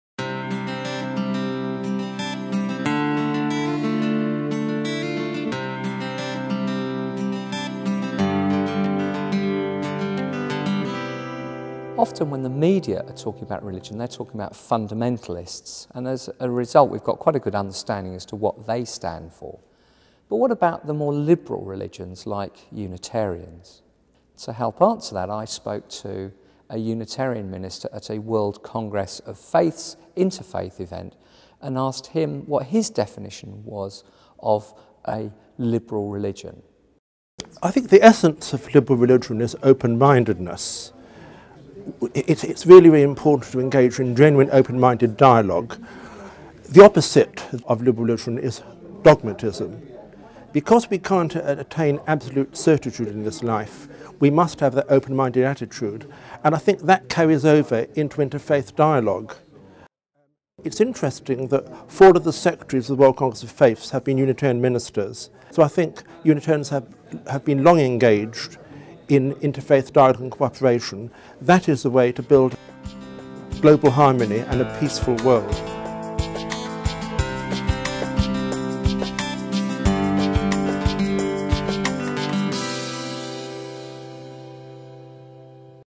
Understanding Unitarians: What is a Liberal Religion?
Rev Feargus O'Connor (Minster of Golders Green and St Albans Unitarians) talking about what makes a liberal religion.